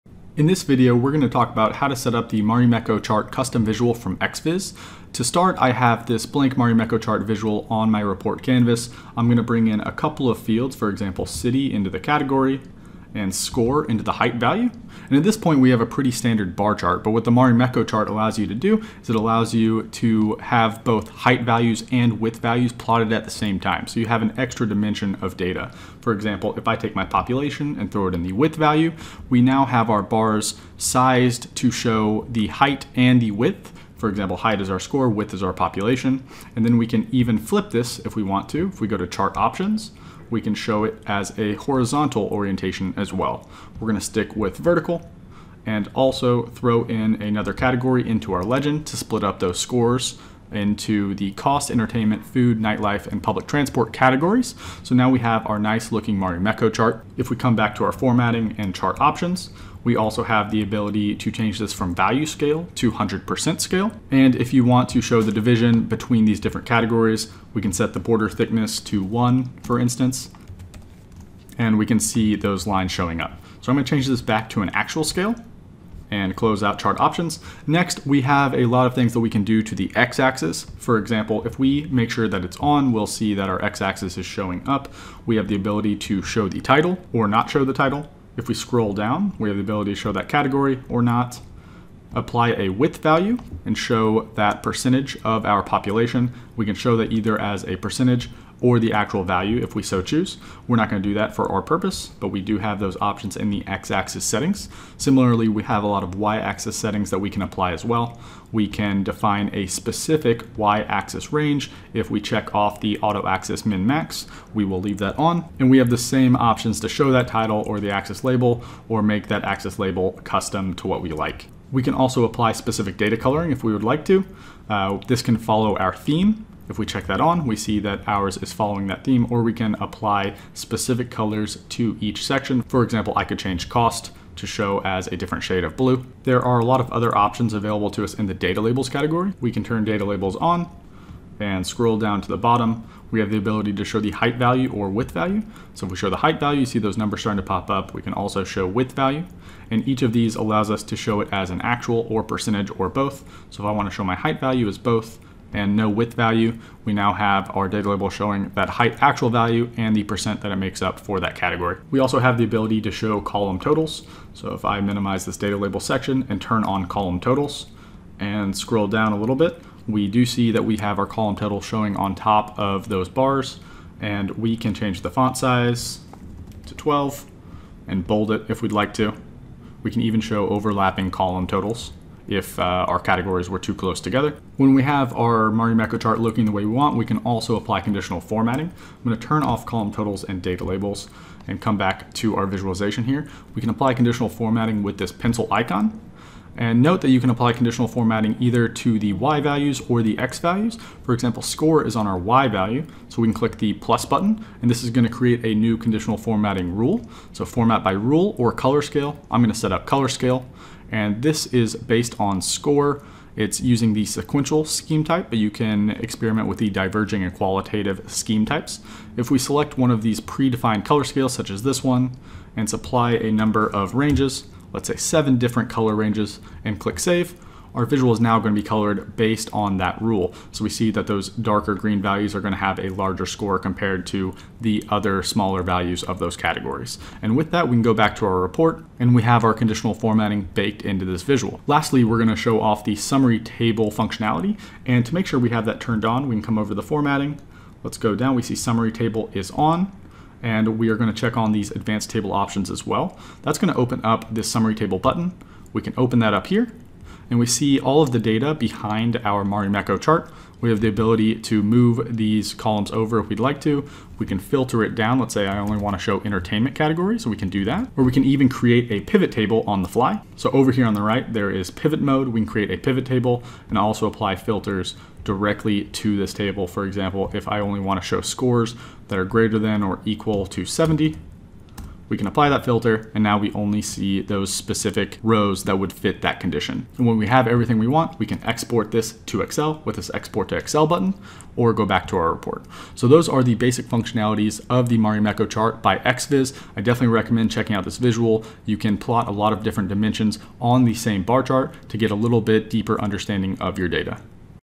Key Features of Mekko Chart - The Power BI Custom Visual
In this video, we discuss how xViz Marimekko Chart for Microsoft Power BI, enables you to visualize categorical data over two percentage scale variables.

The Treemap/Mekko Custom Visual allows you to encode two values (e.g. Revenue & profit margin) along its width and height of each dimension (e.g. product) or a combination of two dimensions (e.g. Products in New York). Configuration options include conditional formatting for the height and width values, and series type formatting of percentage and percentage stacked.

**FEATURES**

Some of the other notable features are as follows:

1. Conditional Formatting
2. Data label Customization
3. Axis Label Customization
4. Display Column Totals for Each Stack
5. Switch Between Chart Types - Horizontal & Vertical Orientation

**ADDITIONAL INFORMATION**

Read more about Mekko Charts for Power BI in our latest blog

ADDITIONAL INFORMATION**

Read more about Advanced Trellis (Small Multiples) in our latest blog

Fallen in love with xViz?